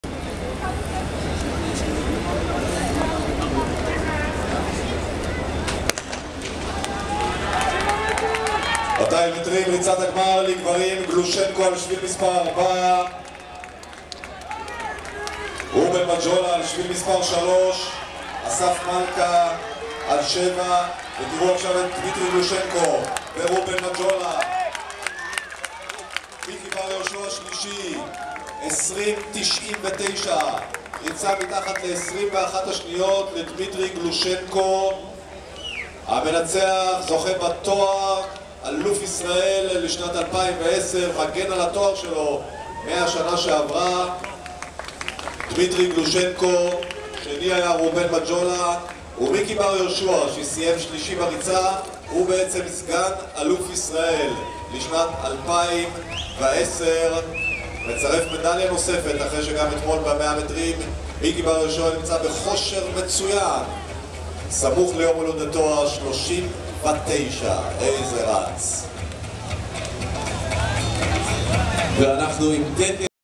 200m - Men
Israel track and field championships - 2nd day. Dimitry Gloushenko is the Israeli champion for the 200m run in the year 2010.